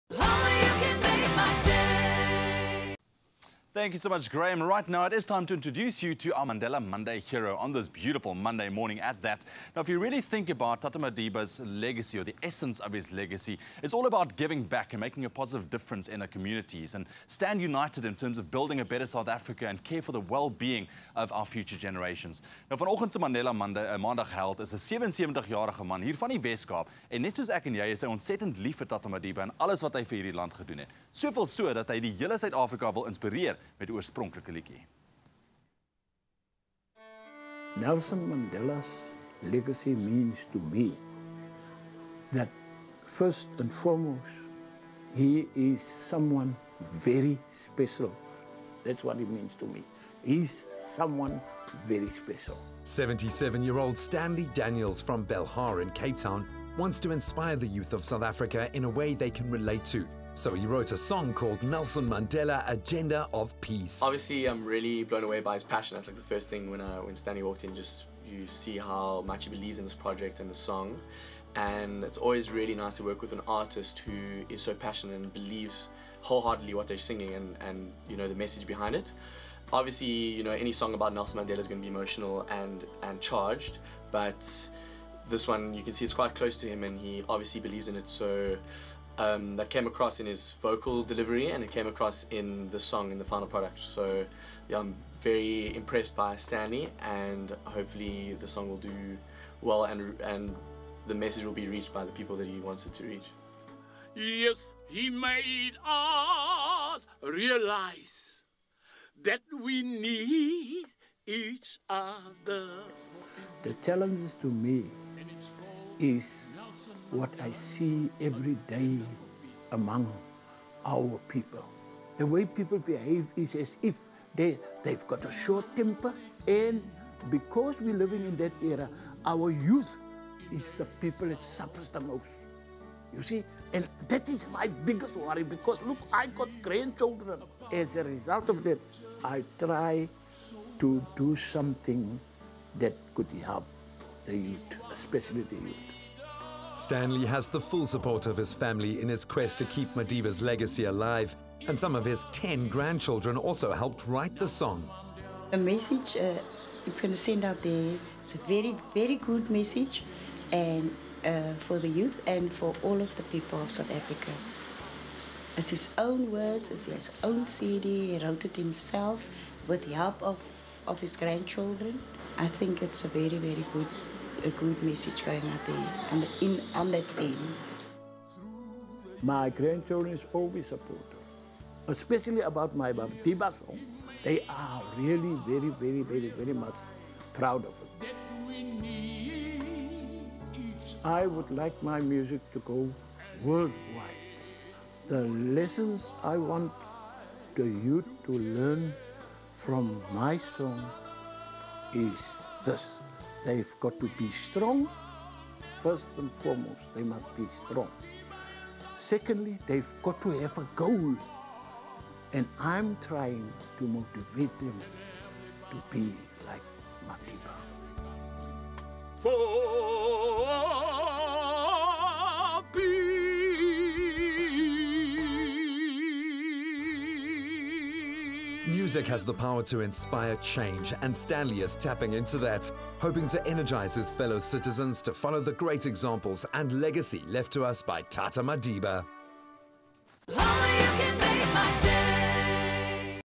Mandela Monday Stanley Daniels– Spreading Madiba’s Legacy
Our Mandela Monday Hero shows us how he’s using his aspirations of becoming a musician to spread Madiba’s legacy!